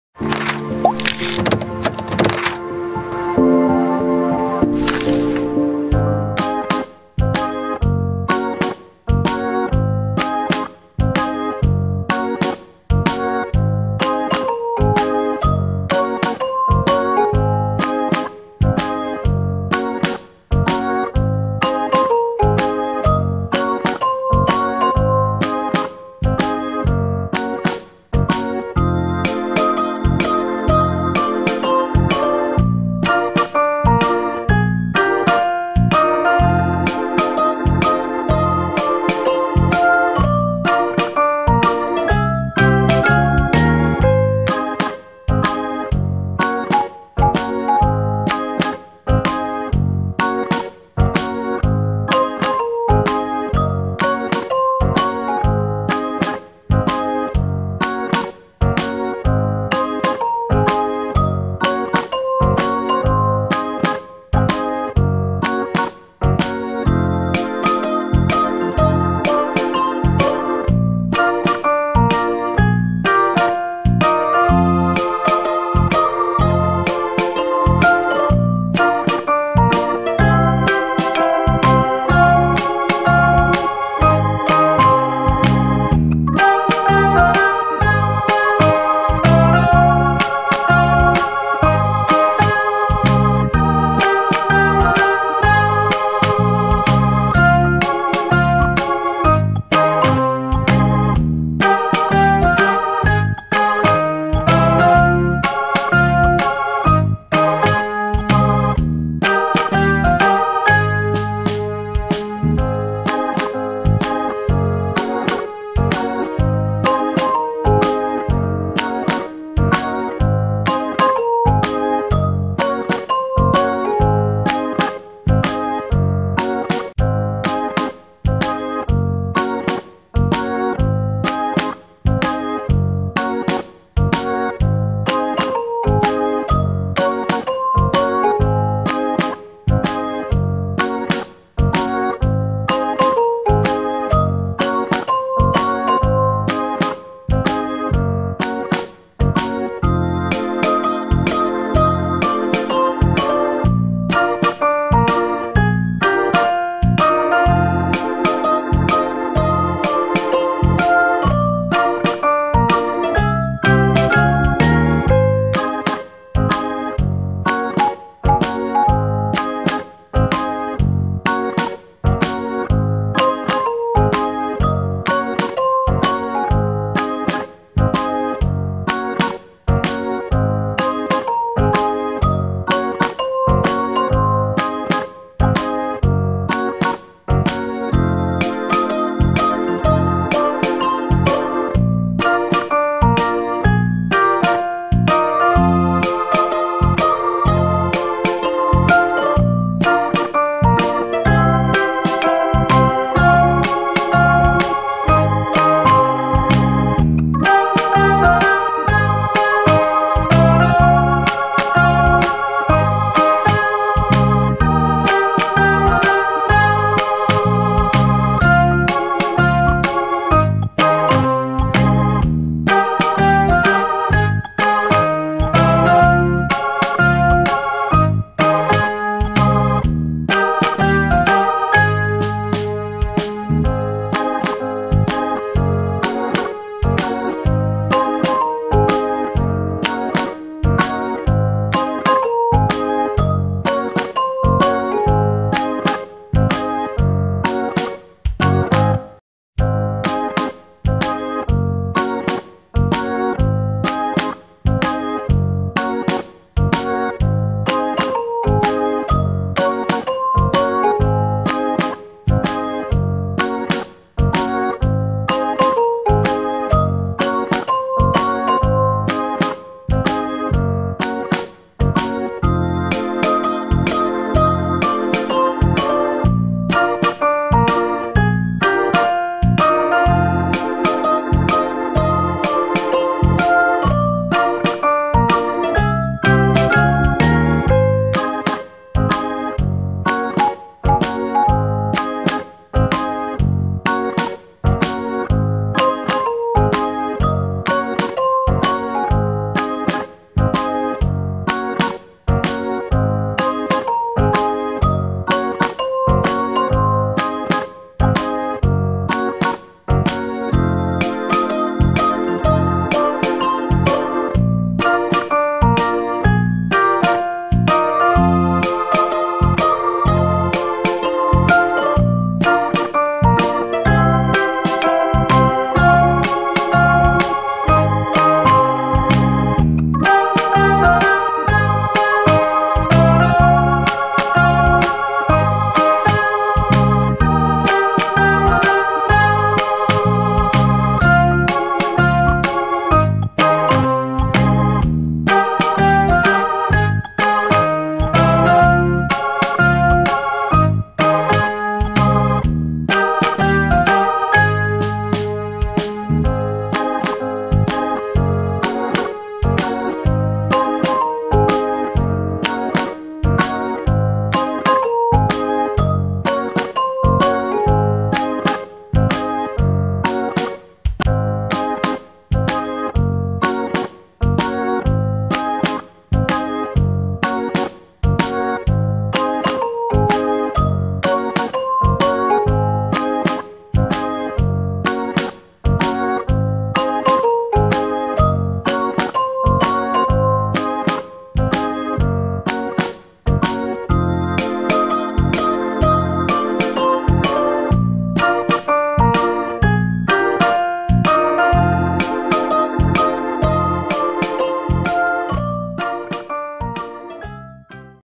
LENGUA DE GATO RECIPE | Paano Gumawa ng Crumble Free Lengua De Gato Cookies | Kasosoyo Sa NEgosyo
Ingredients
1 bar or 1 cup softened butter (not melted)
1/2 cup white sugar
1/2 tsp vanilla
3 large egg whites
1 and 1/2 cup sifted all purpose flou